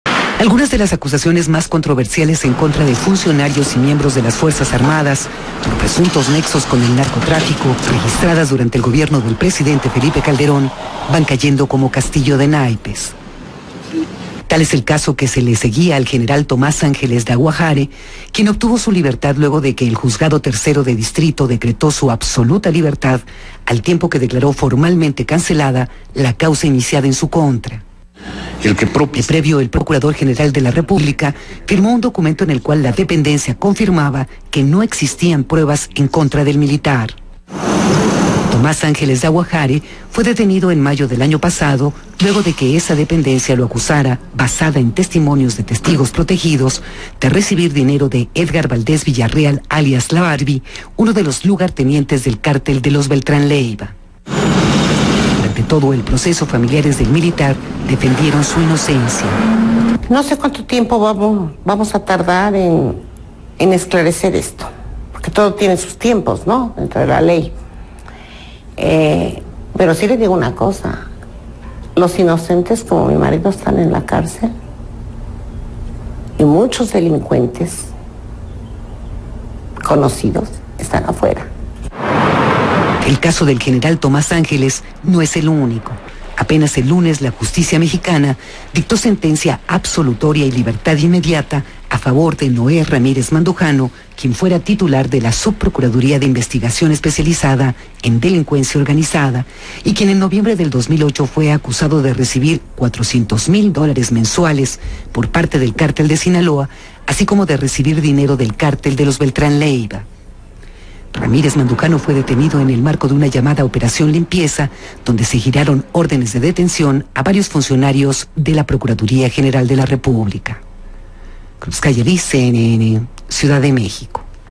General Tomás Ángeles Dauahare sale de prisión
El general en retiro Tomás Ángeles Dauahare salió la noche de este miércoles del penal de máxima seguridad del Altiplano, en el Estado de México, luego de que un juez ordenara su "absoluta e inmediata libertad"